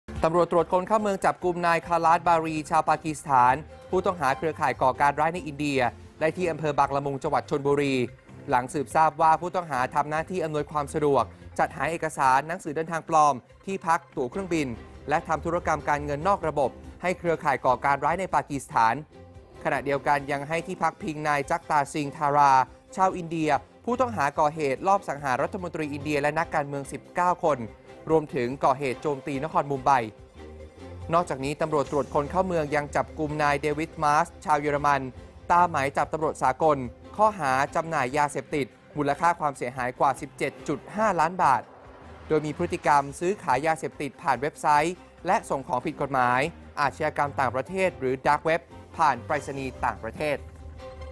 ตำรวจ ตม.จับชาวปากีสถานให้ที่พักเครือข่ายก่อการร้าย
ตำรวจตรวจคนเข้าเมืองจับชาวปากีสถานให้ที่พักพิงผู้ต้องหาก่อเหตุลอบสังหาร รมต.อินเดีย รวมถึงก่อเหตุโจมตีนครมุมไบ พร้อมจับกุมชาวเยอรมนี ผู้ต้องหาค้ายาเสพติดผ่านเว็บไซต์ มูลค่าความเสียหายกว่า 17.5 ล้านบาท